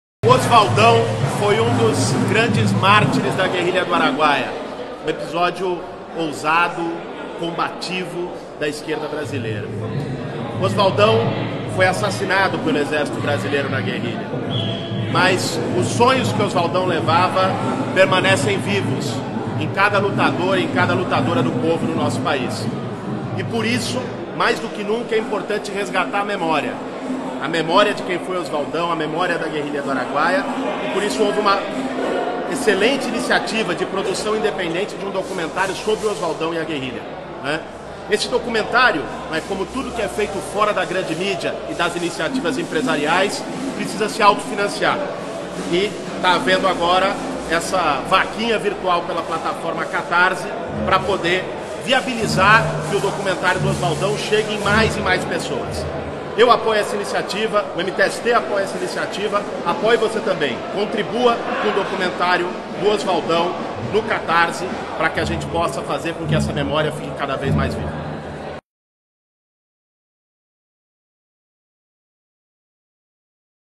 Guilherme Boulos apoia Osvadao nos cinemas
A campanha "Leve o Osvaldão para os Cinemas" tem surpreendido pelo ineditismo de garantir que os temas da esquerda e das lutas democráticas cheguem as salas de cinema, garantindo audiência para um filme que nada tem de comum com os blockbusters de Hollywood. Personalidades dos movimentos sociais, como Guilherme Boulos, resolveram se manifestar em favor da campanha de arrecadação.
"O Osvaldão foi um dos grandes mártires da Guerrilha do Araguaia, um episódio ousado e combativo da esquerda brasiliera.
O Osvaldão foi assassinado pelo exército brasileiro na guerrilha, mas os sonhos que o Osvaldão levava, permanecem vivos em cada lutador e cada lutadora do povo no nosso país. Por isso, mais do que nunca, é importante resgatar a memória. A memória de quem foi o Osvaldão e a memória da Guerrilha do Araguaia.
Por isso, houve uma excelente iniciativa de produção independente sobre o Osvaldão e a Guerrilha. Esse documentário, como tudo que é feito fora da grande mídia e das iniciativas empresariais, precisa se autofinanciar. Está havendo, agora, essa vaquinha virtual pela plataforma Catarse, para poder viabilizar que o documentário do Osvaldão chegua em mais e mais pessoas.
Eu apoio essa iniciativa, o MTST apoia essa iniciativa, apoie você também! Contribua com o documentário do Osvaldão no Catarse para que a gente possa fazer com que essa memória fique cada vez mais viva."